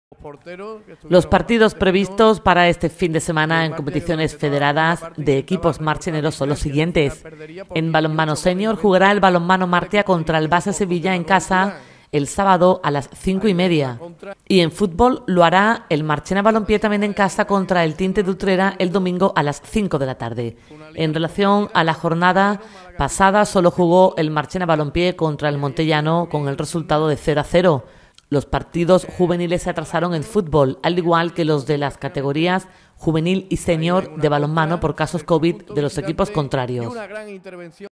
Competición federada fútbol y balonmano
PUBLICA RTVMARCHENA

Los partidos previstos para este fin de semana en competiciones federadas de equipos marcheneros son los siguientes:

En Balonmano Senior, jugará el Balonmano Martia contra el Base Sevilla en casa el sábado a las 17;30h

Y en Fútbol, lo hará el Marchena Bpié también en casa contra el Tinte de Utrera el domingo a las 17;00

En relación a la jornada pasada, sólo jugó el Marchena Bpié contra el Montellano con el resultado de 0 a 0

Los partidos juveniles se atrasaron en fútbol, al igual que los de las categorías juvenil y senior de balonmano por casos covid de los equipos contrarios.